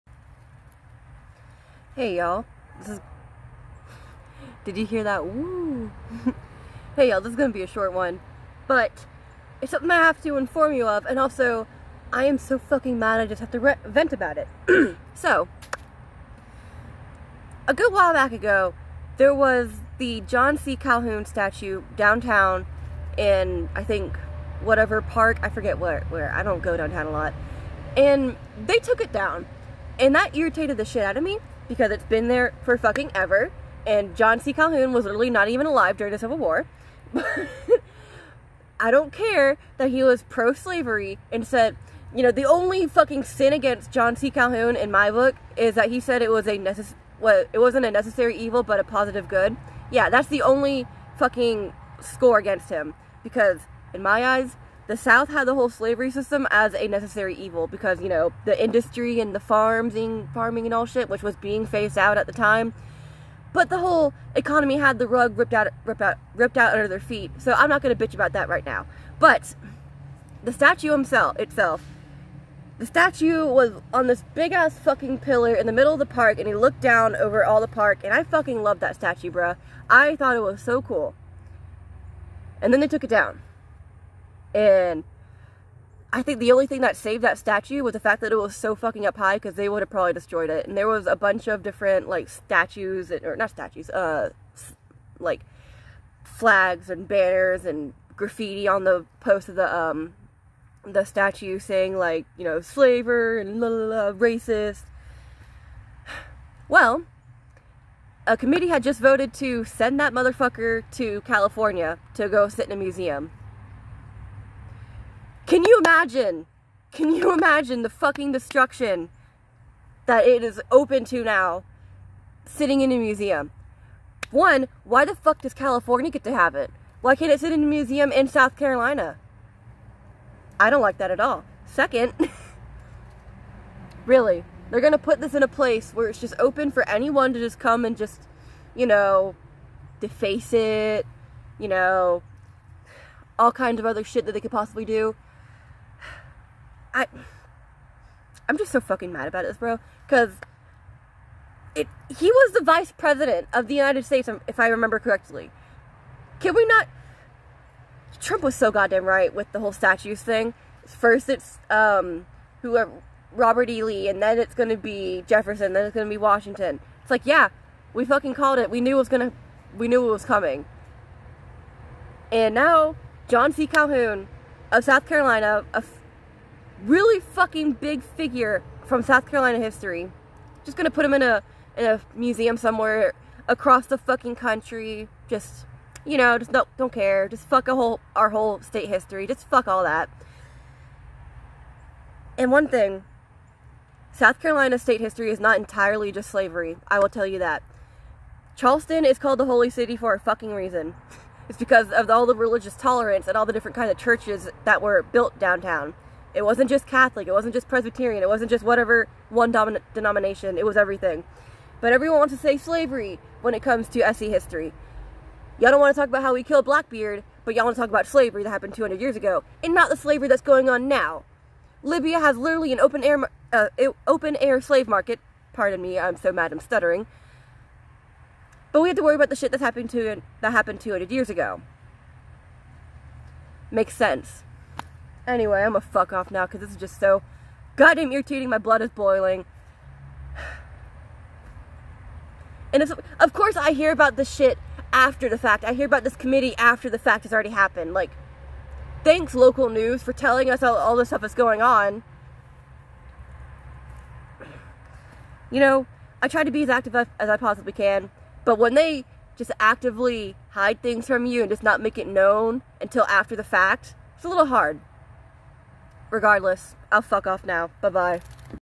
Local John C. Calhoun statue to be sent to California. WHY?! 🤬
my bangs look good today!!
but yeah wtf?!?!

Also on Odysee!